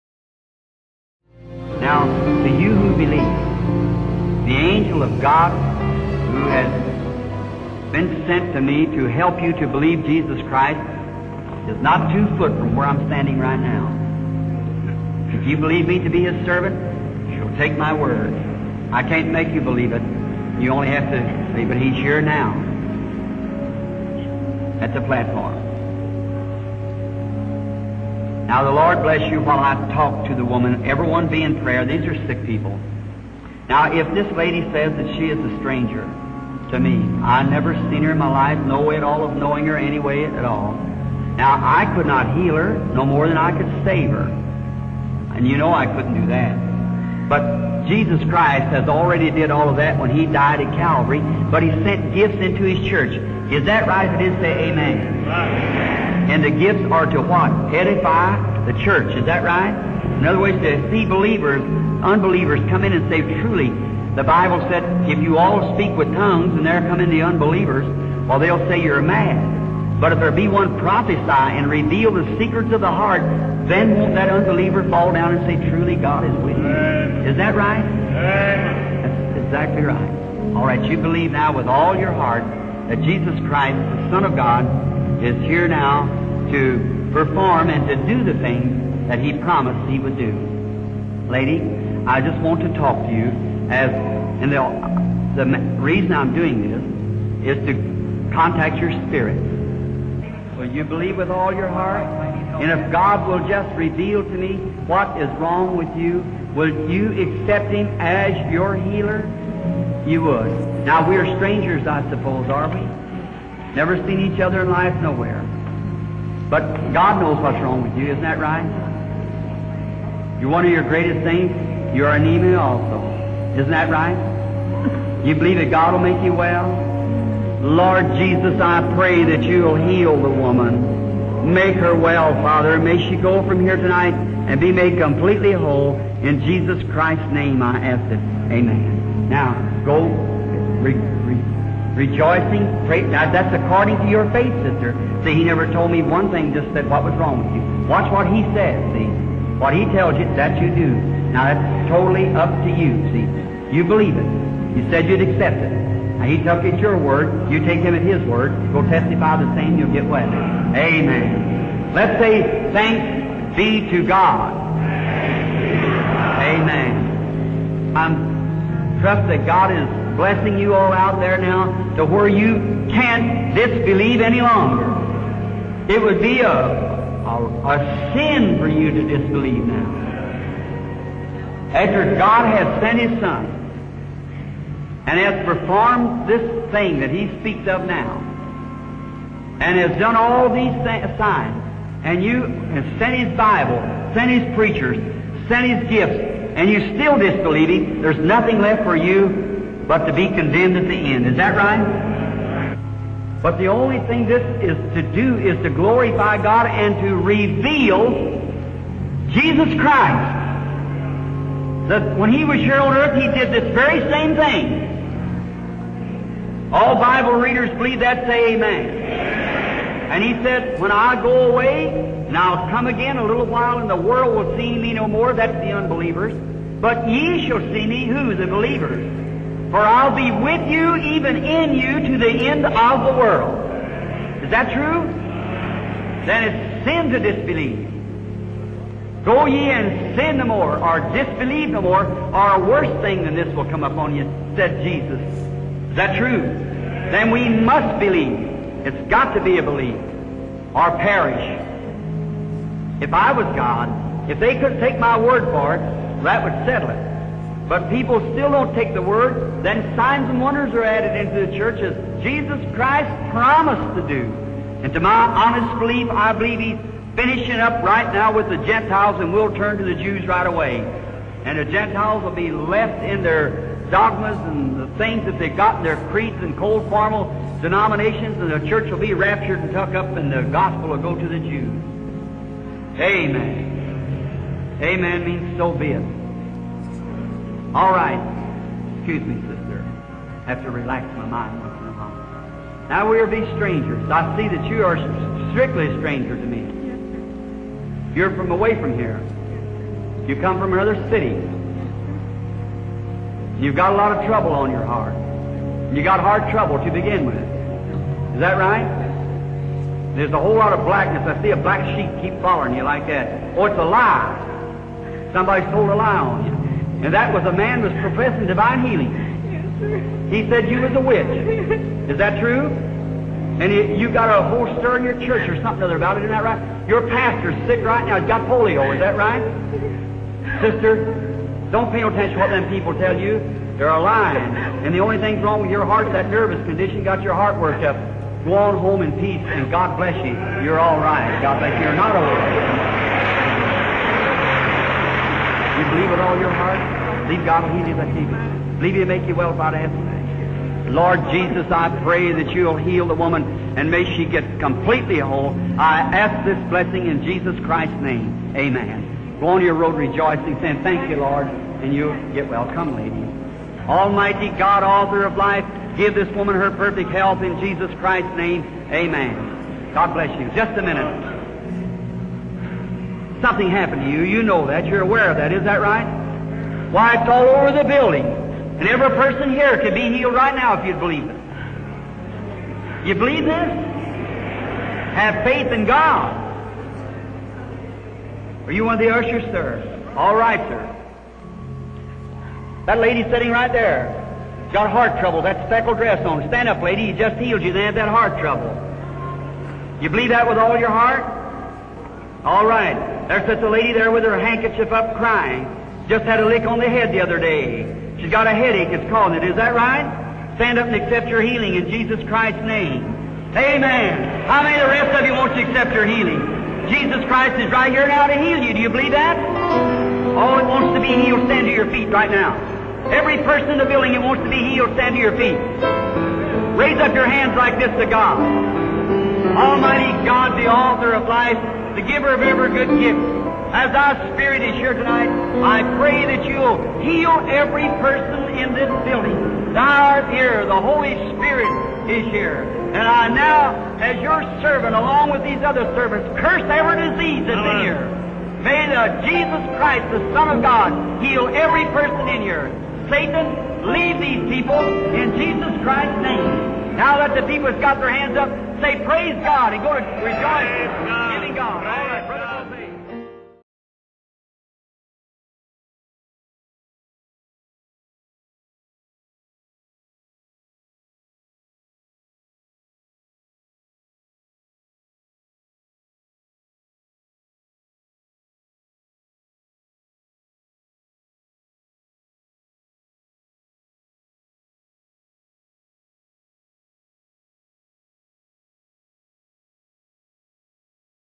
William Branham Miracles [Chicago Crusade Rare] 🔥
William Branham Miracles [Chicago Crusade Rare]. Prophet William Branham carried a powerful anointing for miracles and prophecy. As one of God's Generals he was mightily used by God during the Voice of Healing era.